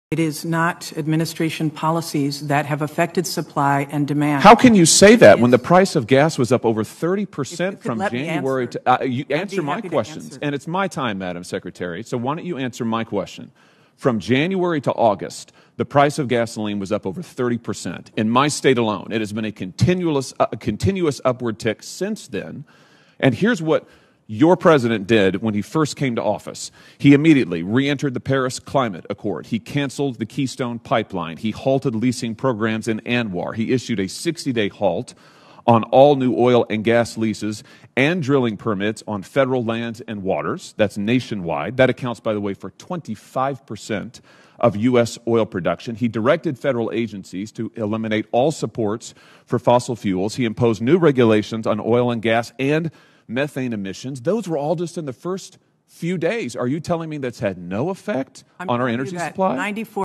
Hawley Questions Biden’s Energy Secretary
In this captivating video, Senator Josh Hawley (R-MO) confronts Energy Secretary Jennifer Granholm over the trading of stocks by her agency’s staff. The intense exchange took place during a Senate hearing on the Energy Department’s annual budget. Senator Hawley’s piercing questions shed light on the ethical concerns surrounding the trading of stocks in companies flagged by ethics officers, including oil giants. The senator’s relentless pursuit of answers showcases his commitment to holding public officials accountable and his dedication to addressing apparent Democratic hypocrisy about fossil fuels.

~ Buddha  : )